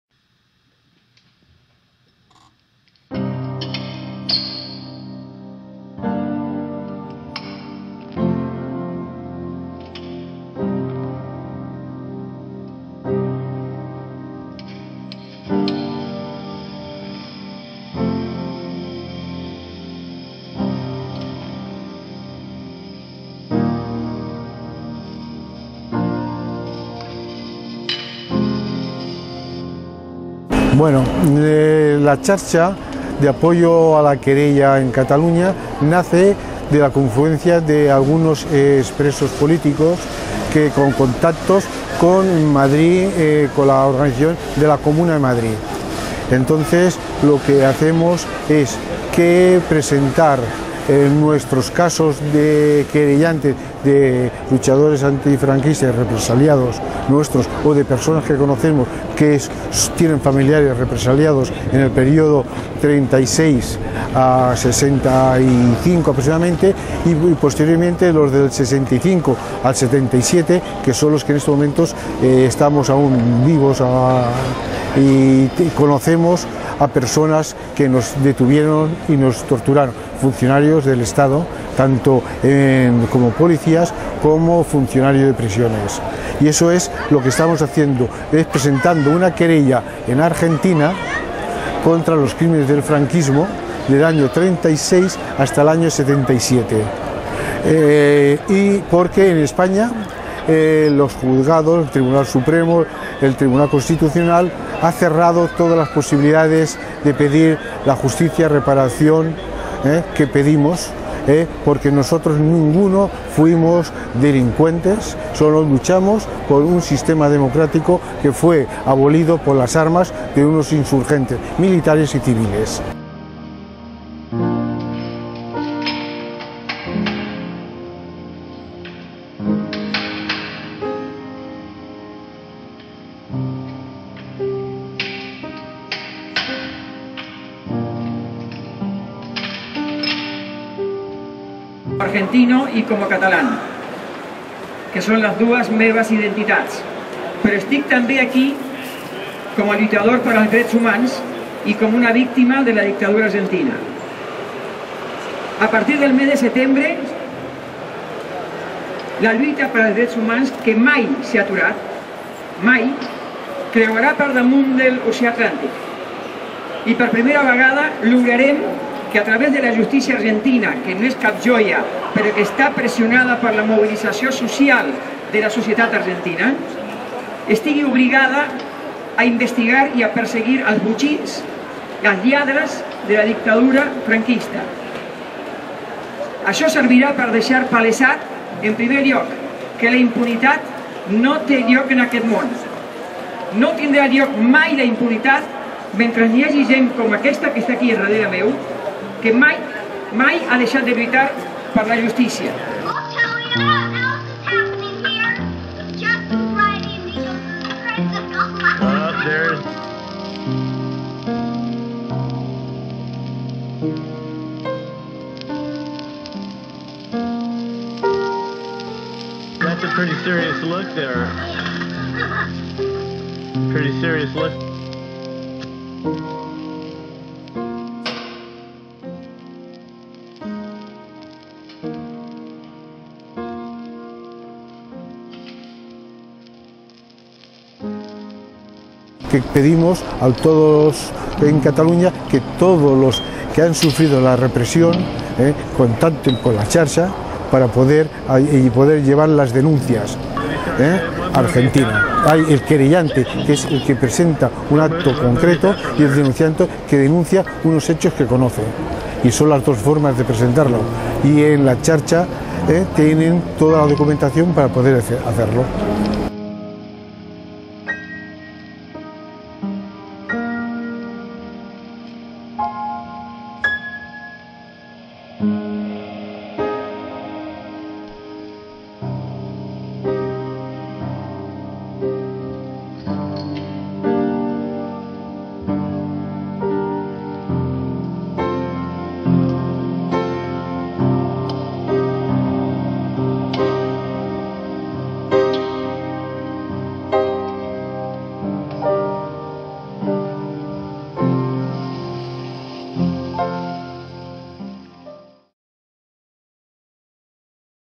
Querella contra el Franquisme
Video resumen del acto celebrado el sábado día 1 de septiembre en la plaza Sant Jaume de Barcelona, referente a la querella que el gobierno argentino presentara contra los crímenes del franquismo.